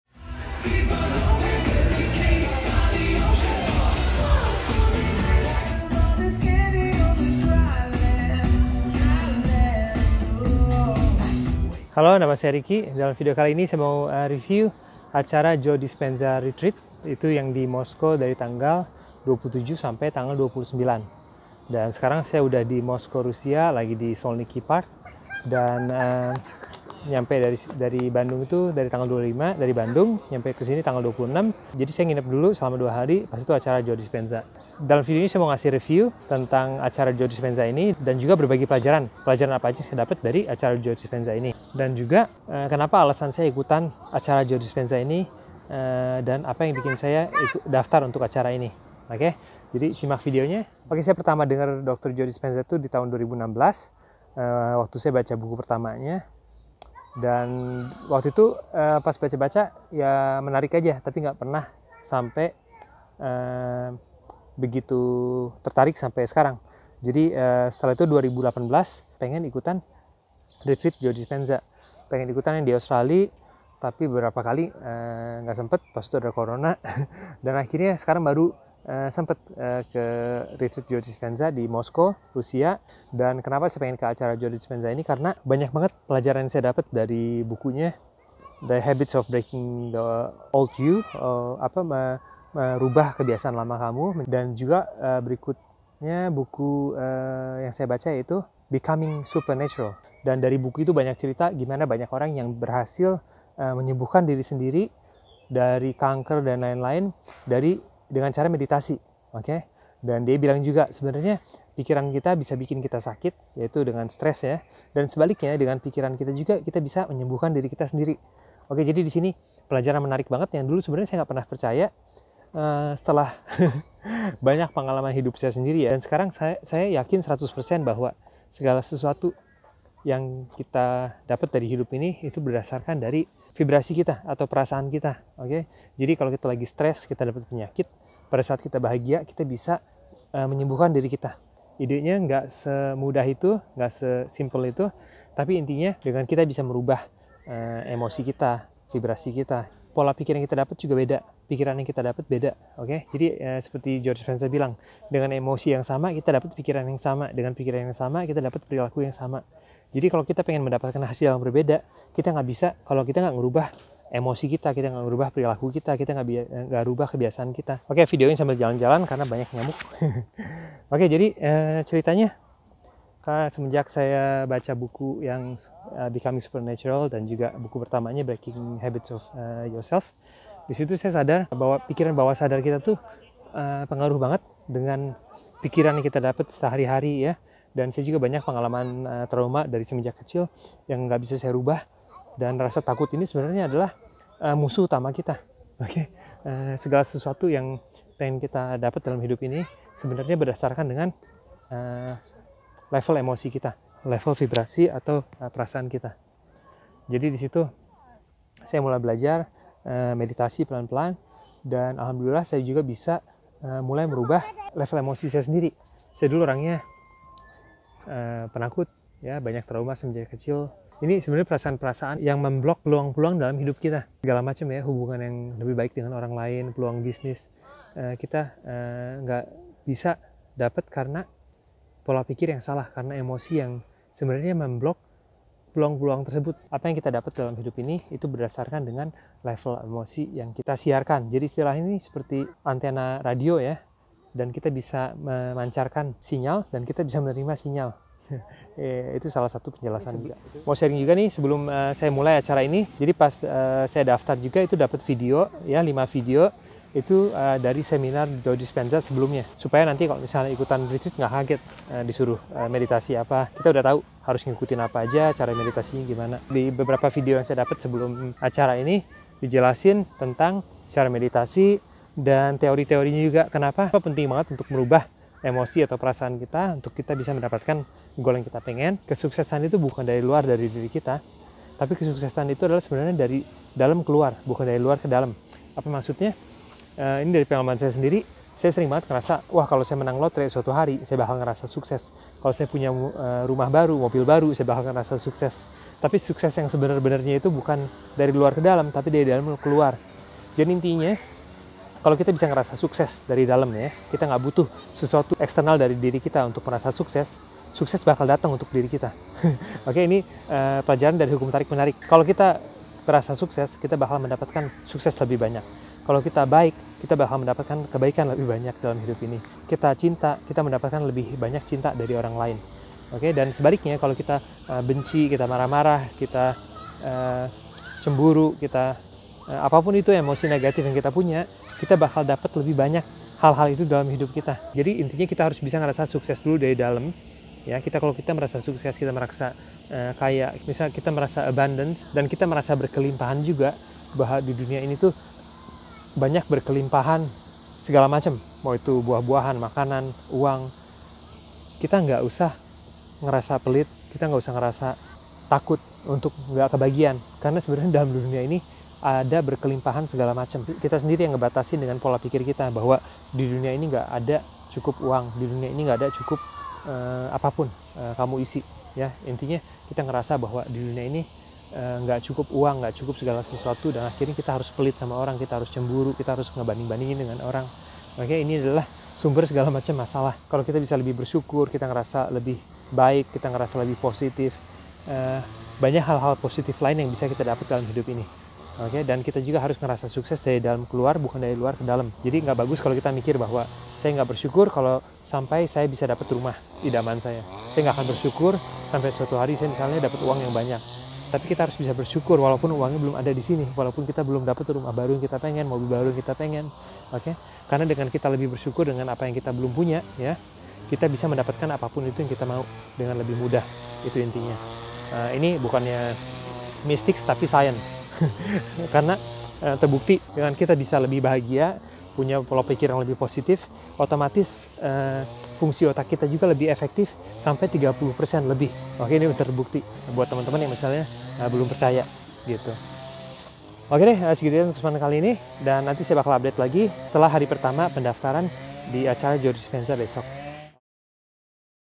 Dr Joe Dispenza Retreat Vlog1
Dalam video kali ini saya berbagi beberapa pelajaran yang saya dapat dari acara Joe Dispenza Progressive Retreat di Moscow, Russia

Untuk kelas diet dan cara menghitung makronutrien kamu bisa join di sini